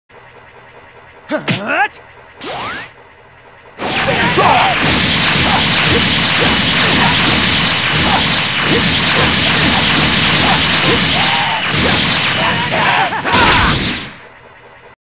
Spirit Ball
Given by Yamcha Mentor who can be found on the path towards the world tournament area